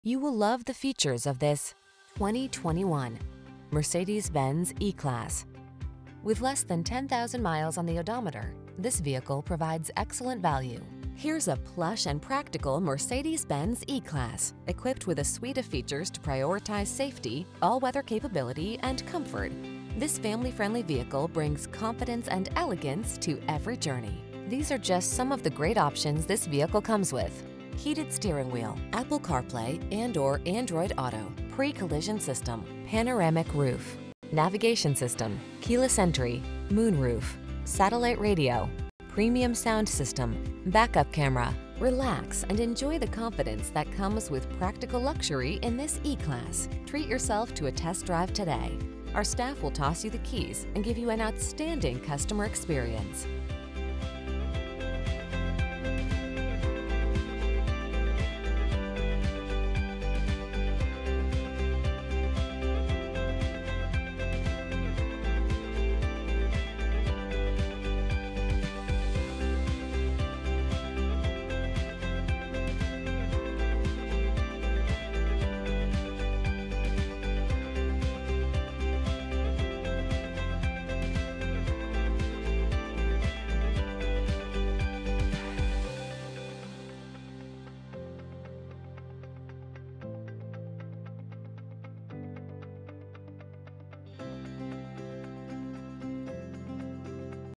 2021 Mercedes-Benz E-Class CT Greenwich, Stamford, Darien, Port Chester, White Plains
Pre-Owned 2021 Mercedes-Benz E-Class E 450 available at Mercedes-Benz of Greenwich located in 261 West Putnam Ave, Greenwich, CT, 06830. Servicing the Greenwich, Stamford, Darien, Port Chester, White Plains area.
Please mention the stock B002549A when you contact the dealership for this vehicle.